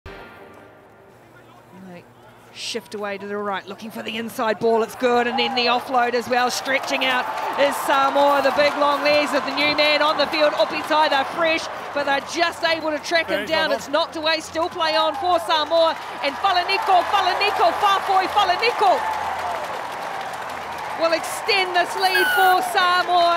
A team effort by Samoa!
A no-look pass, a big hand-off by Motu Opetai and then a diving try by Faafoi Falaniko, it was a team effort for the Samoans in their big win against the Blitzboks at the 2022 HSBC Vancouver Sevens!